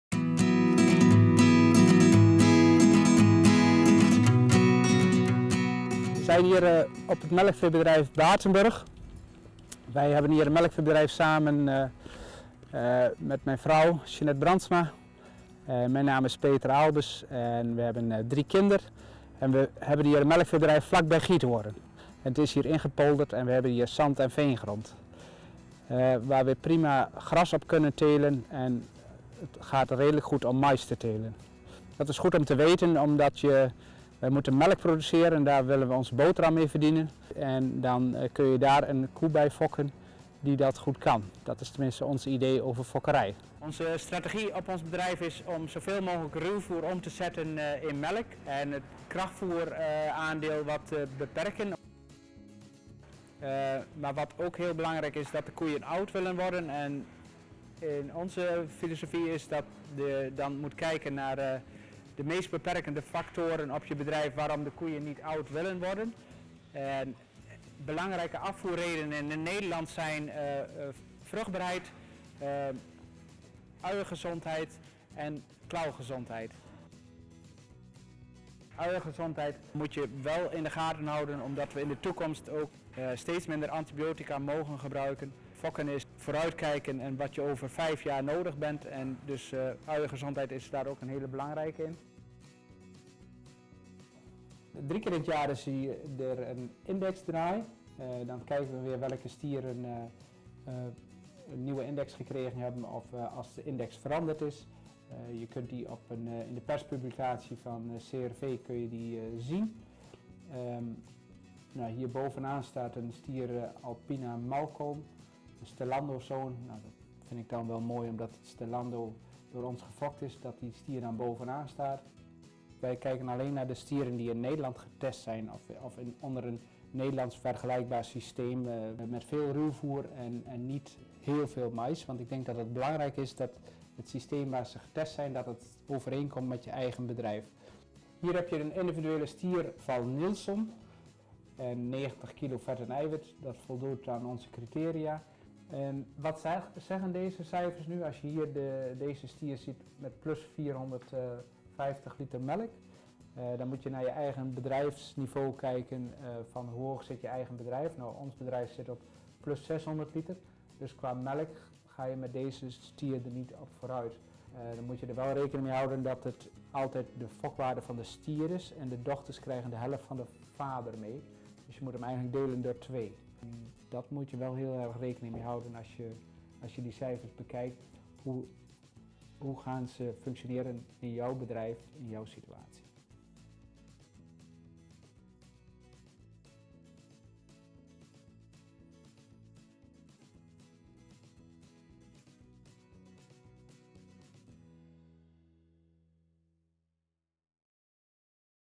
Stierenkeus voor melkveebedrijven
Filmopname bij Batenburg Holsteins, hoe kiest Peter Aalberts de stieren voor de volgende generatie melkkoeien? Deze video is gemaakt binnen het project ‘Ondersteuning CIV Melkveehouderij –2018’, vallend onder de WURKS-regeling.